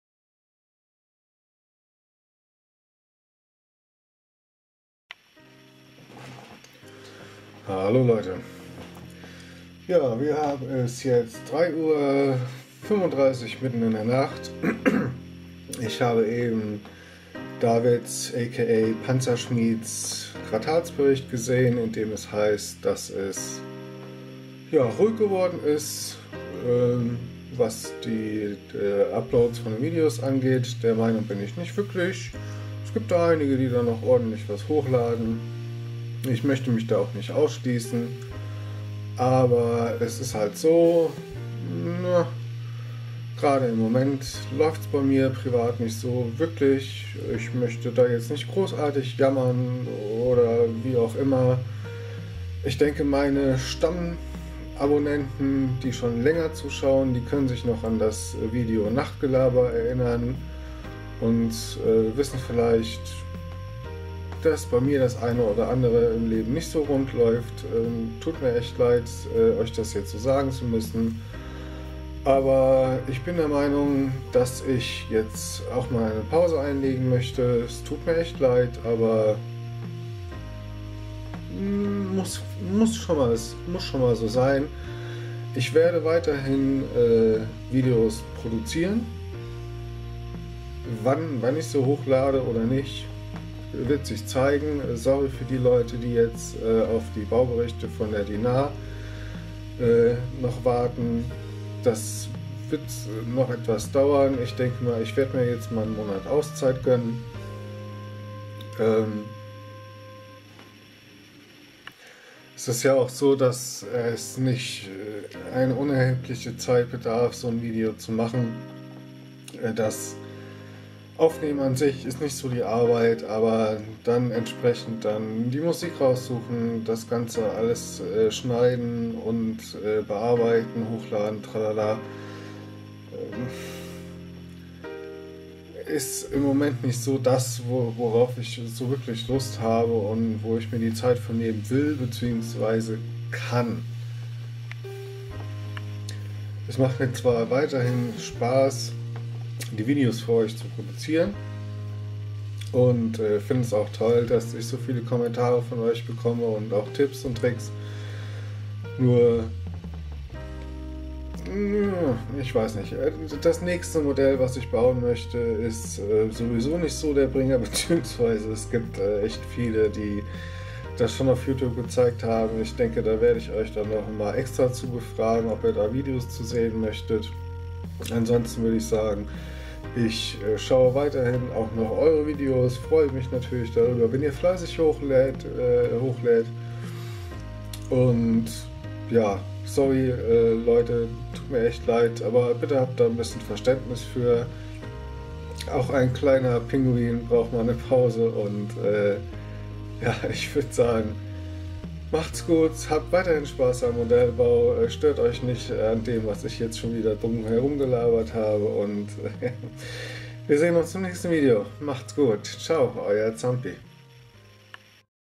Eine kleine Pause
Sorry Leute, aber ich brauche mal eine kurze Pause. Und nein. dass ist kein verspäteter Aprilscherz.